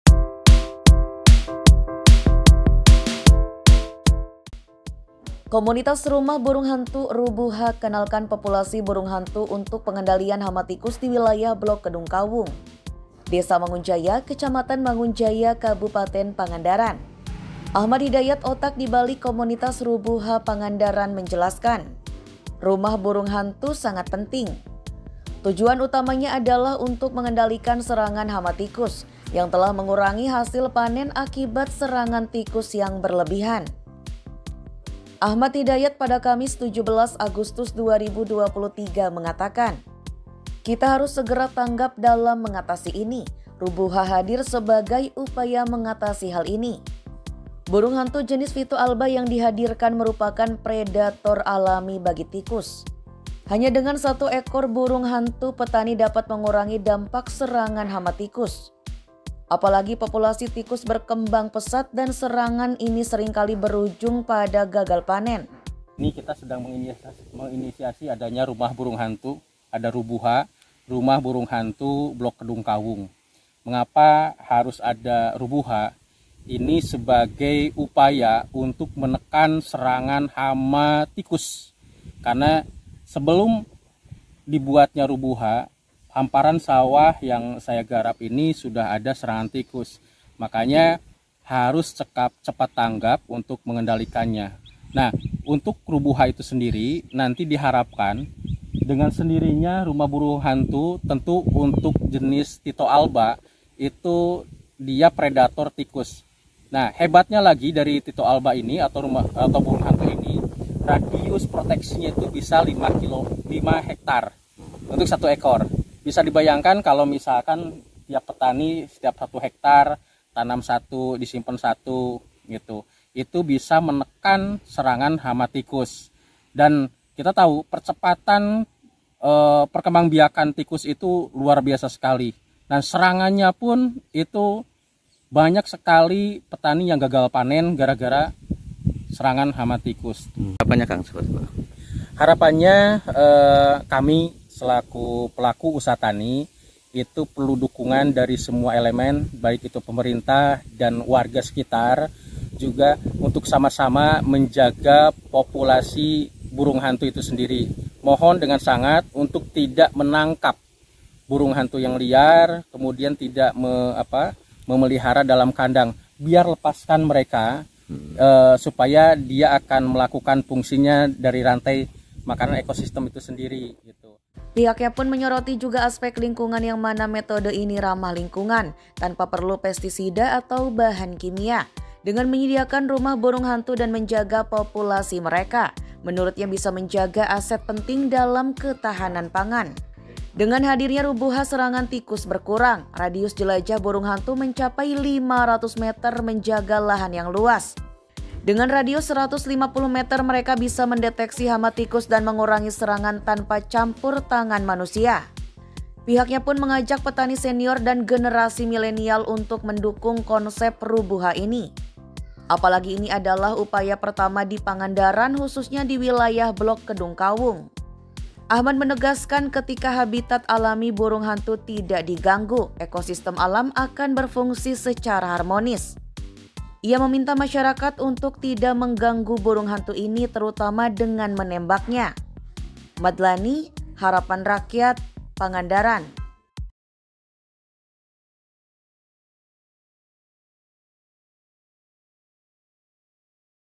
Komunitas Rumah Burung Hantu di Pangandaran Ajak Petani Kendalikan Hama Tikus
Komunitas Rumah Burung Hantu (RUBUHA) kenalkan populasi Burung Hantu untuk pengendalian hama tikus di wilayah Blok Kedungkawung, Desa Mangunjaya, Kecamatan Mangunjaya, Kabupaten Pangandaran.

Wartawan : Madlani
Editor : Samsul
VO : Nita